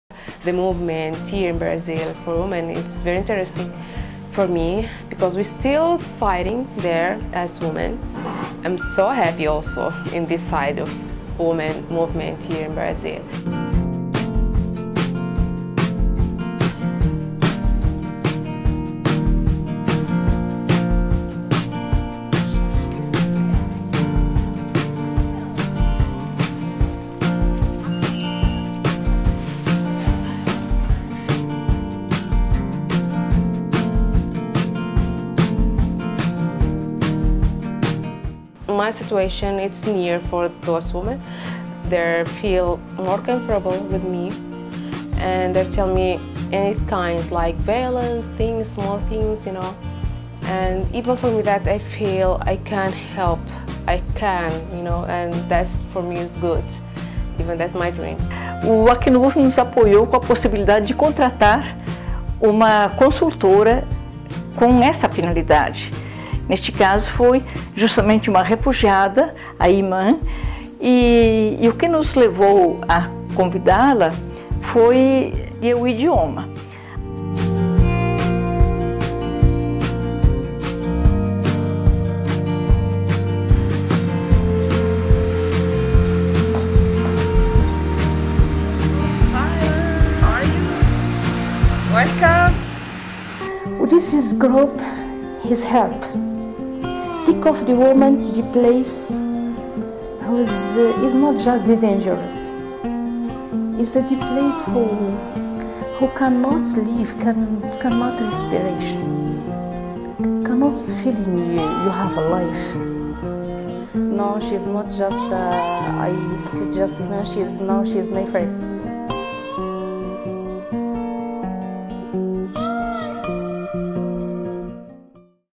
Com experiência humanitária, sudanesa ajuda outras refugiadas a recomeçar a vida no DF
Com experiência humanitária, sudanesa ajuda outras refugiadas a recomeçar a vida no DF e região.

Atuando no projeto “Mulheres Unidas”, o trabalho de Eiman Haru é exemplo da luta pelo fim da violência contra as mulheres refugiadas – e pauta do Dia Laranja pelo Fim da Violência contra as Mulheres, marcado todo dia 25. Iniciativa do Instituto de Migrações e Direitos Humanos (IMDH) tem o apoio da Agência da ONU para Refugiados (ACNUR).

Como grande parte das mulheres forçadas a se deslocar, a trajetória de Eiman é marcada pela superação das dificuldades inerentes ao refúgio e à integração local. Confira nesse vídeo e na reportagem especial